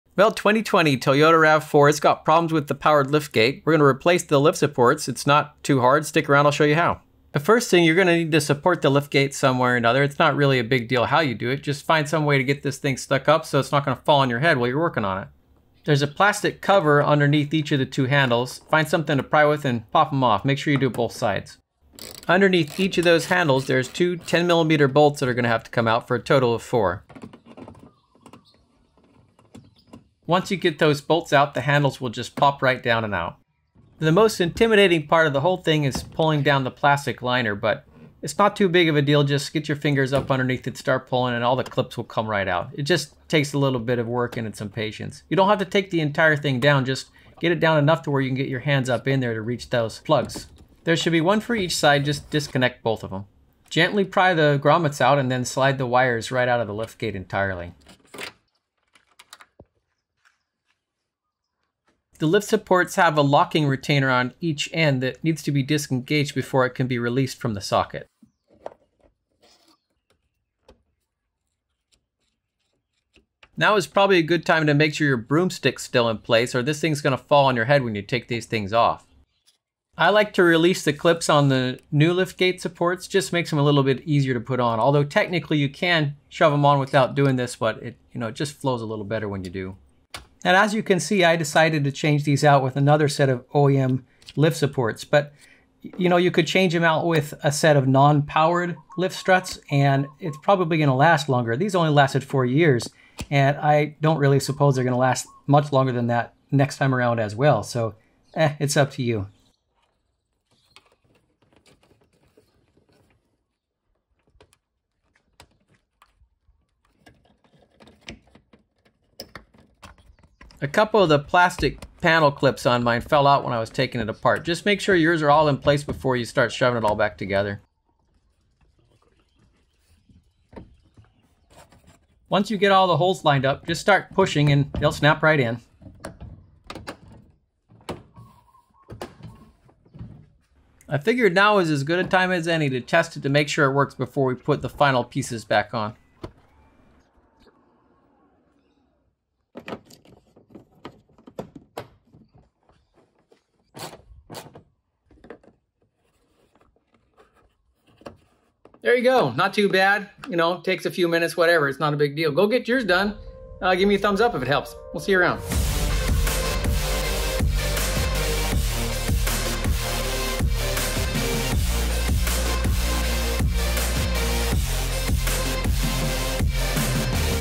Toyota Rav4 Lift Support Replacement
The powered lift gate on this 5th gen Toyota Rav4 was broken and getting jammed.  It's a pretty easy fix with some replacement lift supports.